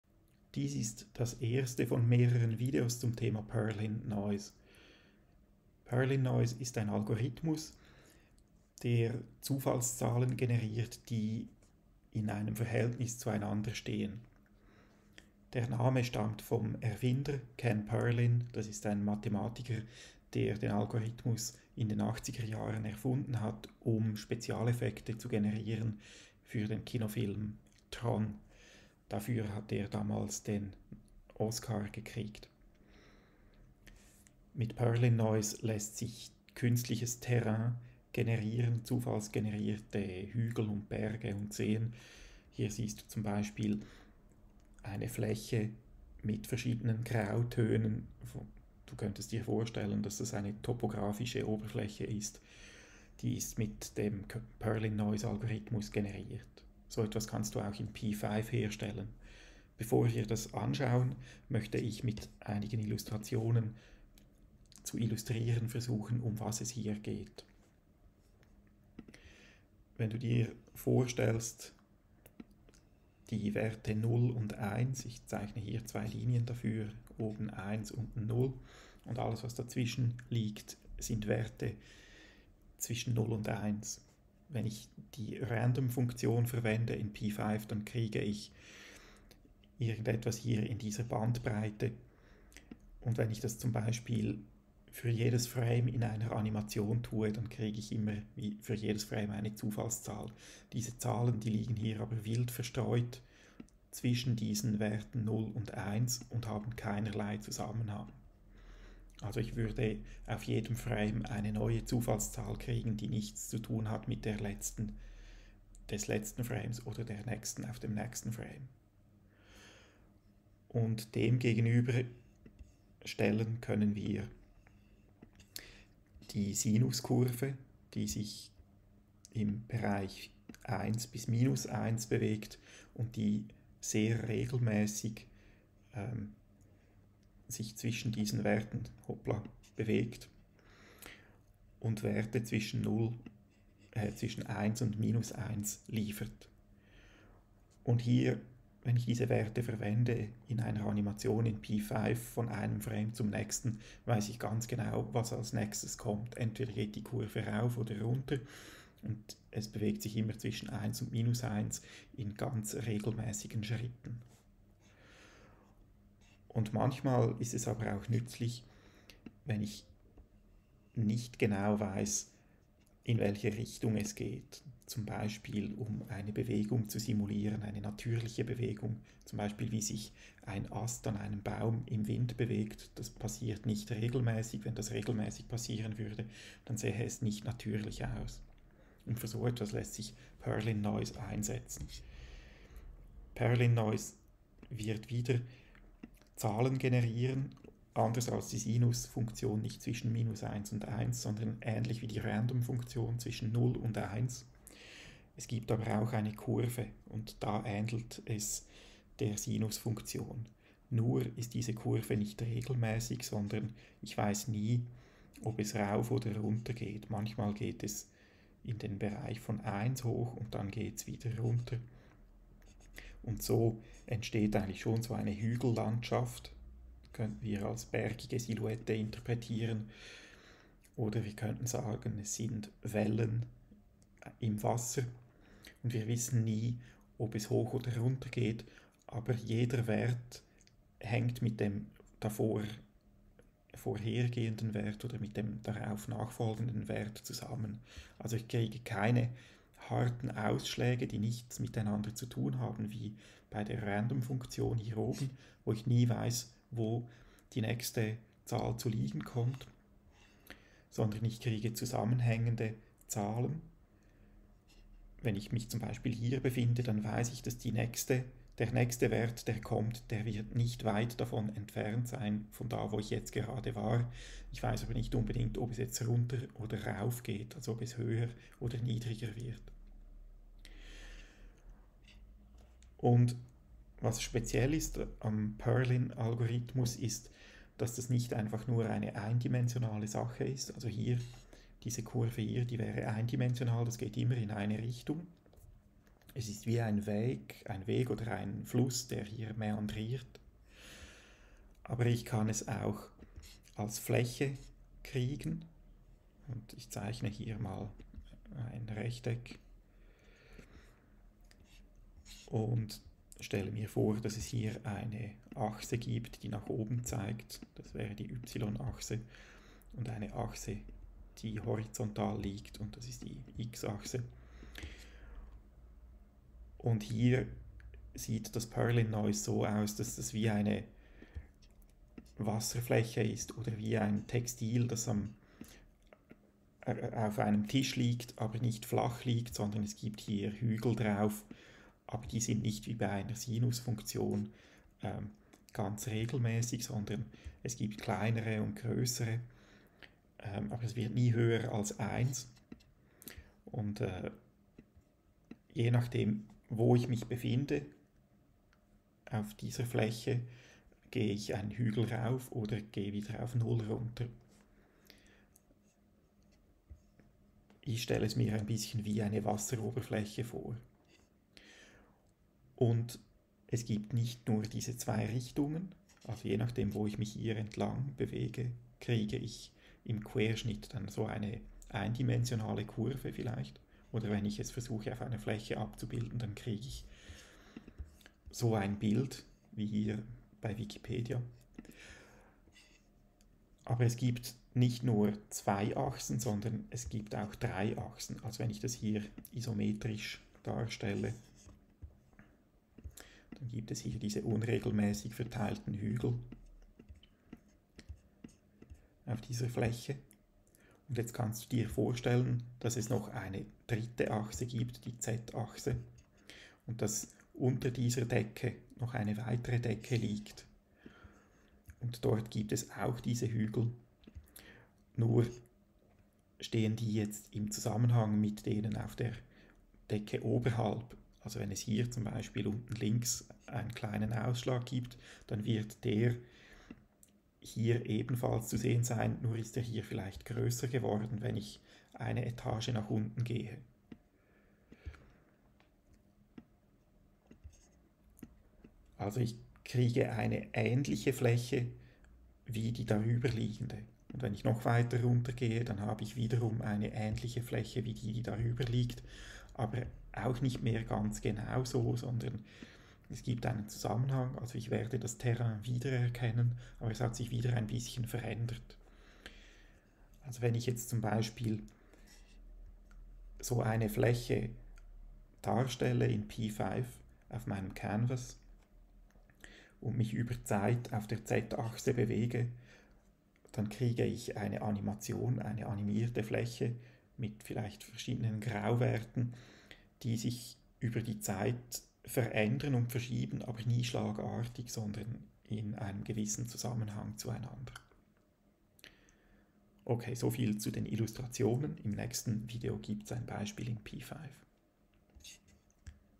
Perlin Noise – Intro
Perlin Noise ist ein Algorithmus, der zufällige Werte zwischen 0 und 1 generiert, die in Zusammenhang zueinander stehen und sich als unregelmässige Kurve abbilden lassen. Perlin Noise wird verwendet, um «naturähnliche» Bewegungen oder Formen nachzubilden.